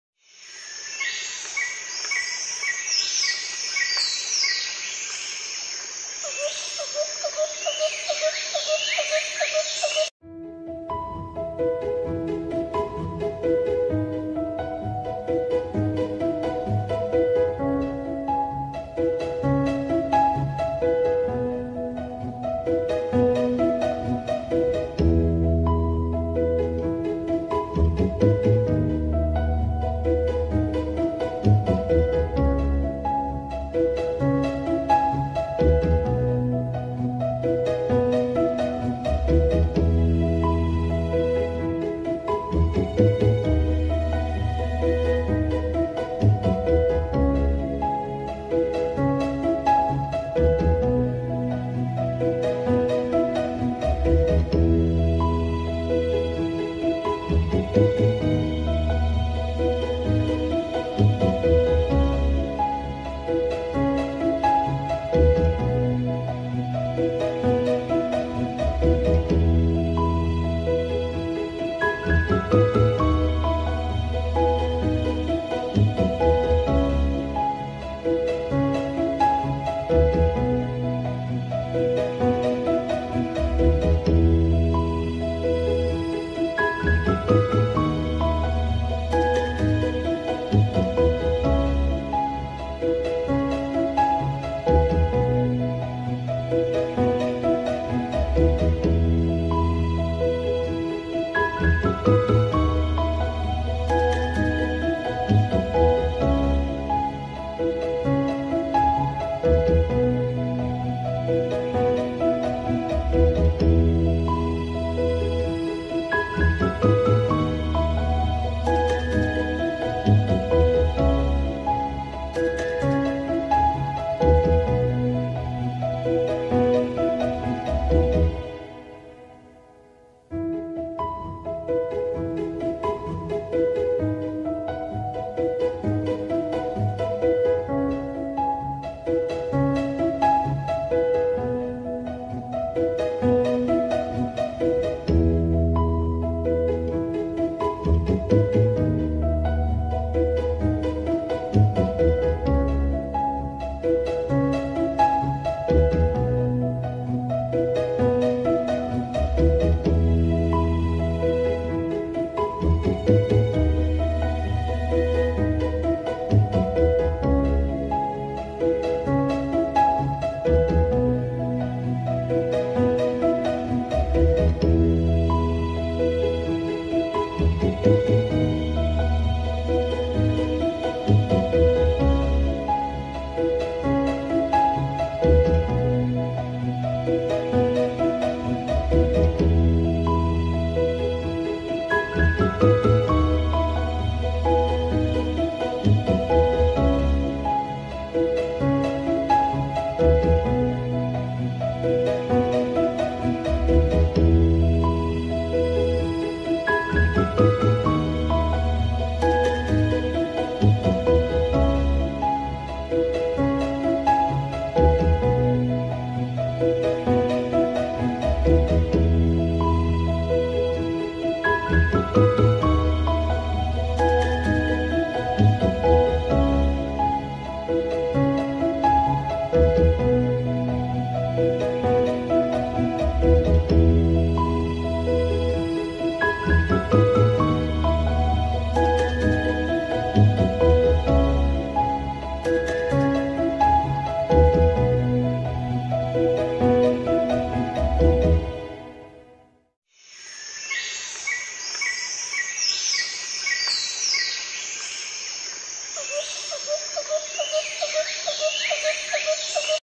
Quando il posto è buono l'oro c'è sempre
Come trovare oro in spiaggia (Sicilia),
con la batea